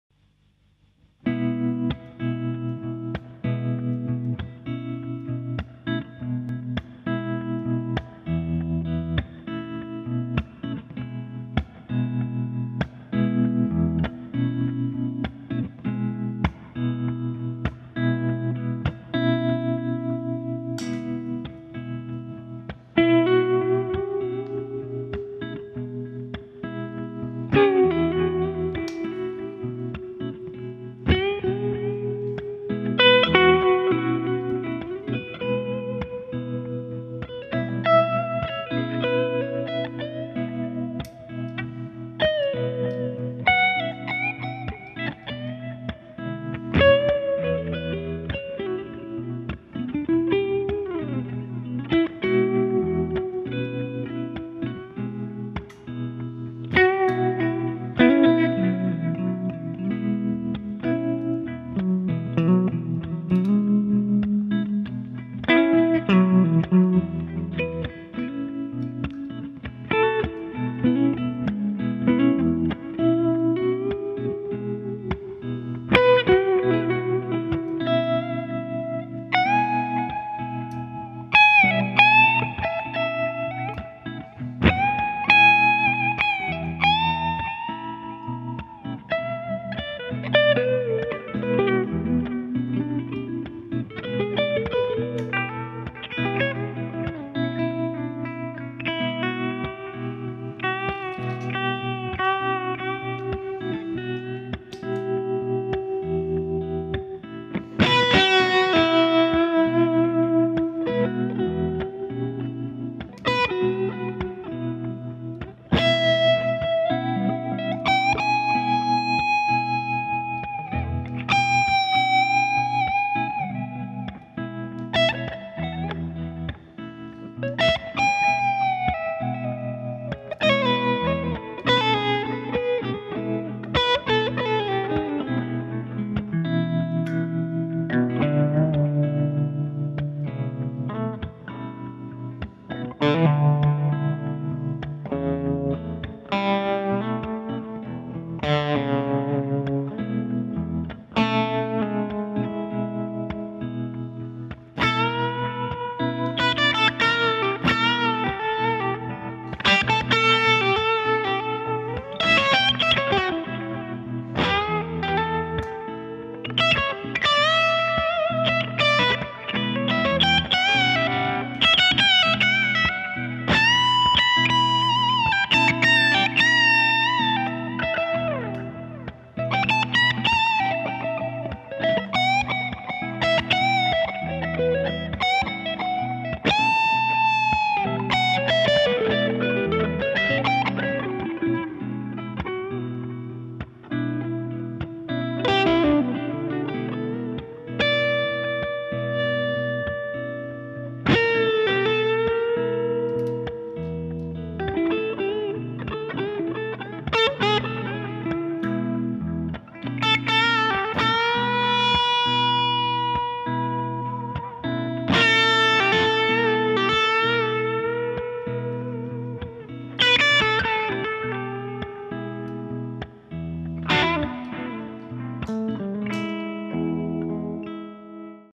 2001 Gibson Les Paul Custom Shop Flametop 1958 Reissue Part3
Gibson Les Paul Custom Shop Flametop (!) 1958 Reissue, Historic Collection, Amber "59 aged" Pickups. Boss RC-2 Looper 2001 Okko Diablo Gain+ Overdrive, Amp: Fender Vibroverb (original 1963), Vahlbruch SpaceTime Delay,